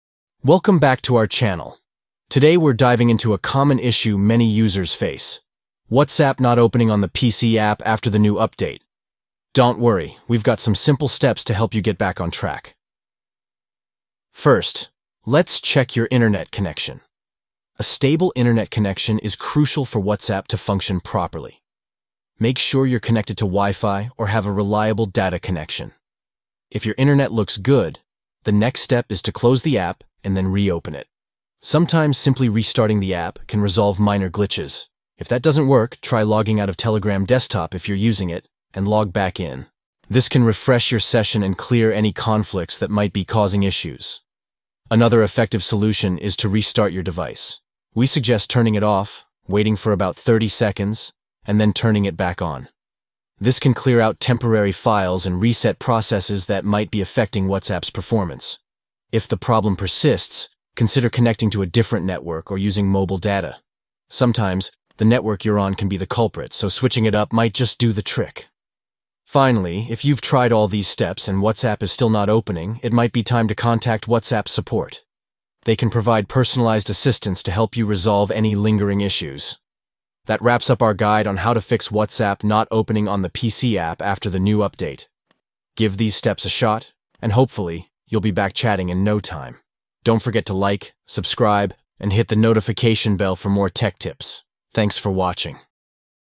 How To Fix WhatsApp Not Opening On PC App (New Update)
Learn how to fix the issue of WhatsApp not opening on your PC app with our step-by-step guide. Whether you’re experiencing problems after a new update or just want to troubleshoot, we've got you covered. Follow these simple steps to get back to chatting with ease:

1. Check your internet connection.
2. Close the app then re-open it.
3. Log out of Telegram Desktop and log back in.
4. Sometimes restarting your device can help. We suggest turning your device off, waiting for 30 seconds, and turning it back on.
5. Try connecting to a different network or using data.
6. Contact WhatsApp support.

Don't let a simple issue stop you from staying connected.